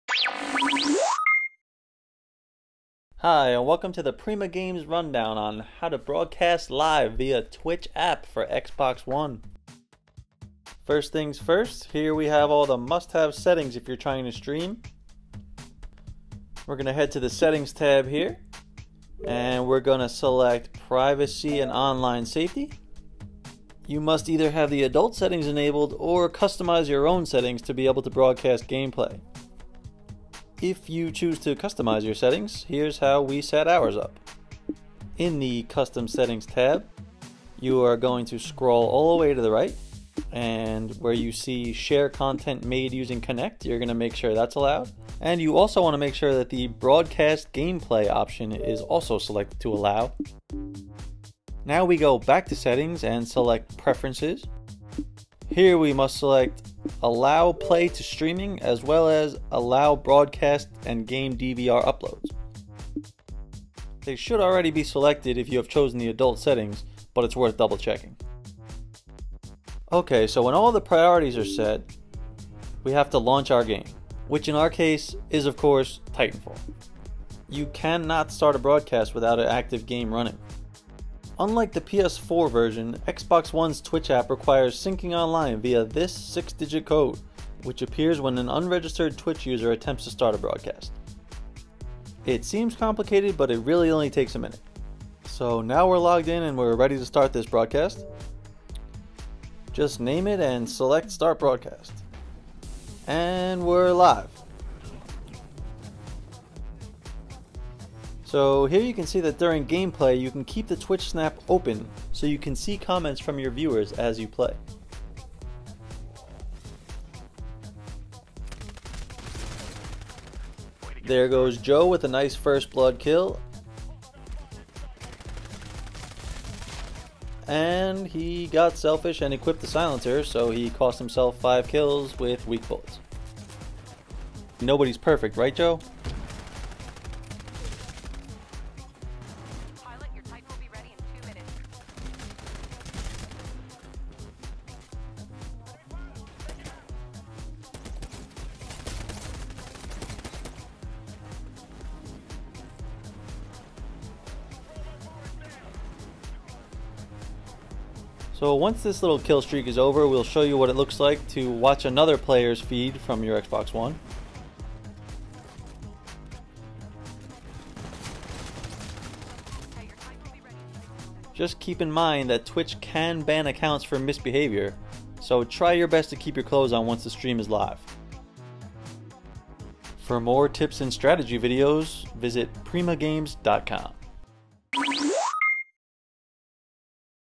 How To Live Stream On XBOX One to Twitch.tv
This Primagames video shows you how to broadcast gameplay from the XBOX One using the Twitch App.